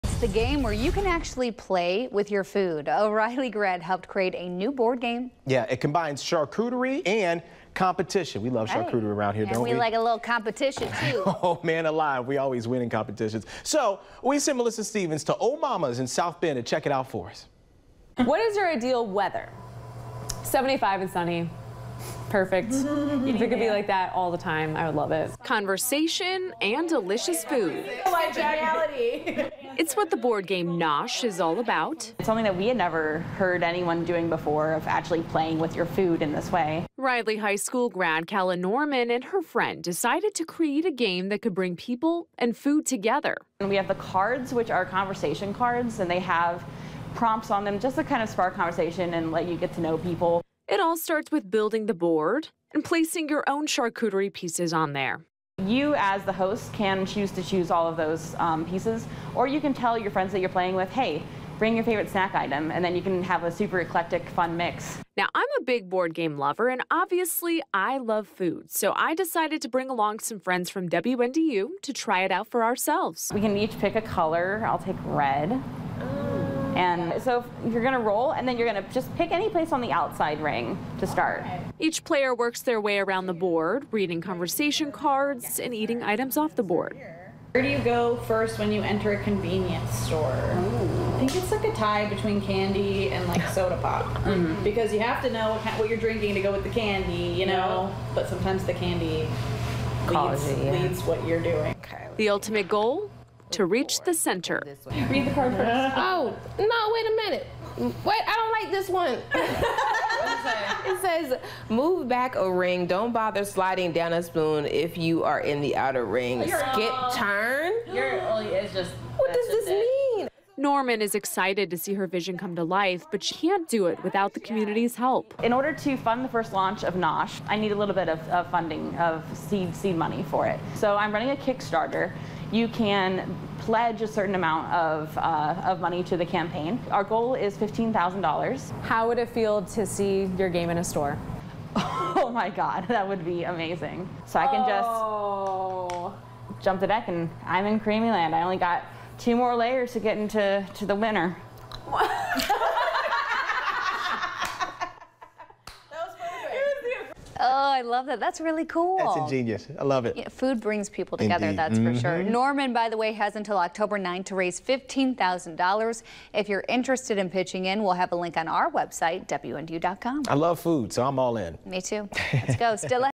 South Bend board game creator seeks funding for new, food-based game
Nosh is a board game that combines charcuterie and competition. But to get it up and running, its creator needs a little help!